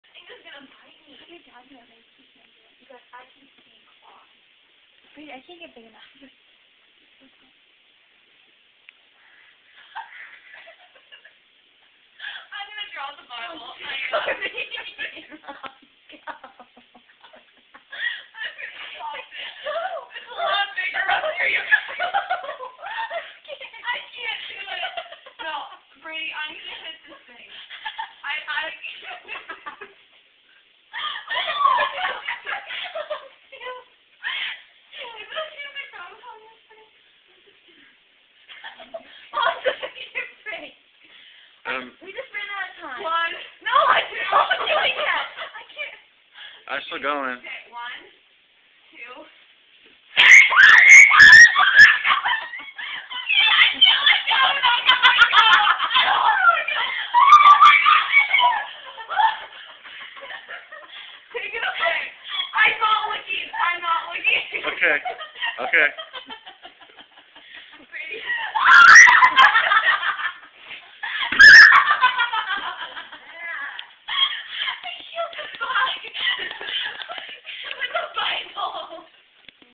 Holy Bug Squashing
Hannah Swanson the bug exterminator and her trusty side kick Shelbie Miller are brought in the deal with a bug "problem" at the Bomber Household. Their weapon... the Bible!